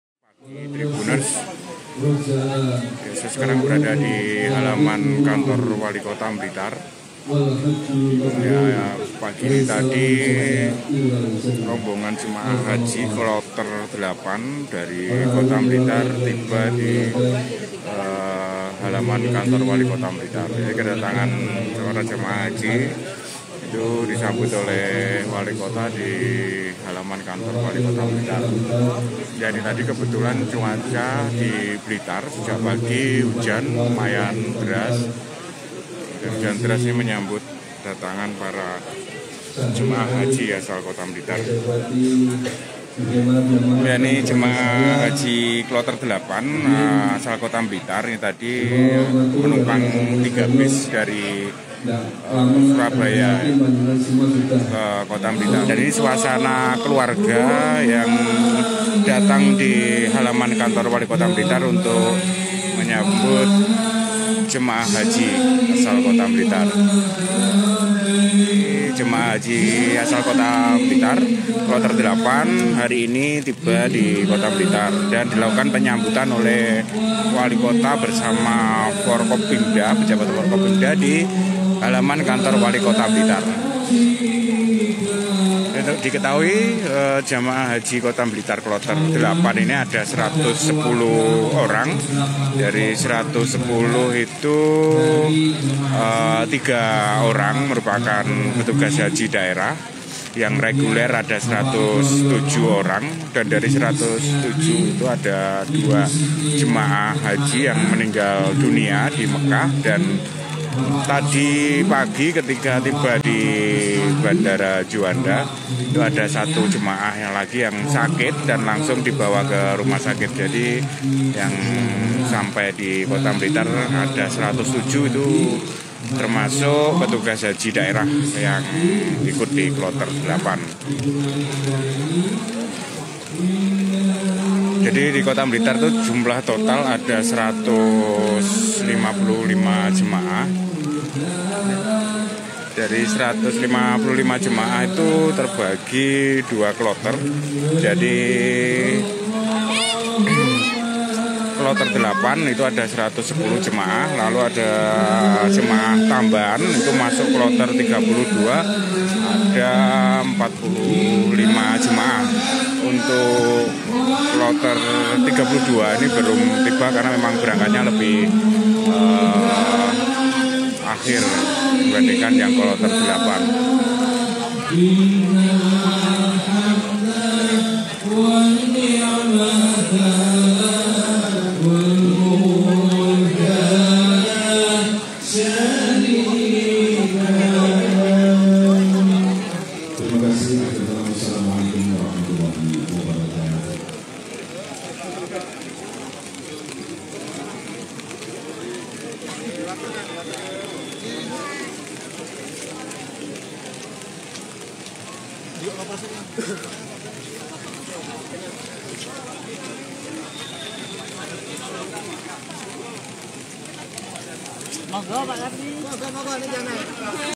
Sempat Terlambat Datang, 107 Jemaah Haji Kloter 8 Akhirnya Tiba di Kota Blitar
vp: didit / reporter: samsul hadi

TRIBUNMATARAMAN.COM | BLITAR - Hujan deras mengiringi kedatangan rombongan jemaah haji kloter 8 Kota Blitar di halaman Kantor Wali Kota Blitar, Jumat (7/7/2023).

Meski kedatangannya sempat terlambat, para keluarga juga terlihat menyambut hangat kedatangan para jemaah haji yang selesai melaksanakan rukun haji selama lebih sebulan di Mekkah.

Sejumlah orang tampak saling berpelukan dengan jemaah haji yang baru tiba di halaman Kantor Wali Kota Blitar.

Wali Kota Blitar, Santoso bersama pejabat Forkopimda Kota Blitar juga ikut menyambut kedatangan rombongan jemaah haji.

"Saya ucapkan selamat datang kepada para jemaah haji yang baru datang. Mudah-mudahan para jemaah yang baru pulang dan sudah menjalankan rukun, sunnah dan wajib menjadi haji mabrur," kata Santoso saat menyambut kedatangan rombongan jemaah haji di halaman Kantor Wali Kota Blitar.

Santoso mengatakan kedatangan rombongan jemaah haji asal Kota Blitar berjalan lancar, meski sempat ada keterlambatan.

Sesuai rencana, rombongan jemaah haji tiba di Kota Blitar pada Jumat (7/7/2023) pukul 07.00 WIB.

Namun, karena ada masalah teknis, rombongan jemaah haji baru tiba di Kota Blitar pada Jumat (7/7/2023) pukul 10.00 WIB.

"Karena faktor teknis, rombongan jemaah haji yang semestinya tiba di Kota Blitar pukul 07.00 WIB, baru datang pukul 10.00 WIB," ujarnya.

Dikatakan Santoso, jumlah jemaah haji kloter 8 Kota Blitar sebanyak 110 orang. Dari total itu, ada dua orang jemaah yang meninggal dunia di Mekkah.

Selain itu, juga ada satu jemaah jatuh sakit dan harus dirawat di rumah sakit saat tiba di Surabaya.

"Jumlah jemaah haji kloter 8 Kota Blitar semestinya 110 orang. Tapi, yang kembali ke Kota Blitar hari ini sebanyak 107 orang. Dua jemaah meninggal di Mekkah dan satu jemaah sakit saat turun dari pesawat dan dirawat di Surabaya. Kita doakan yang sakit segera sembuh," katanya.

Santoso berharap, para jemaah haji yang baru pulang bisa memakmurkan tempat ibadah di lingkungannya masing-masing.

Seperti diketahui, jumlah total jemaah haji asal Kota Blitar sebanyak 155 orang yang terbagi menjadi dua kloter, yaitu kloter 8 sebanyak 110 orang dan kloter 32 sebanyak 45 orang. (sha)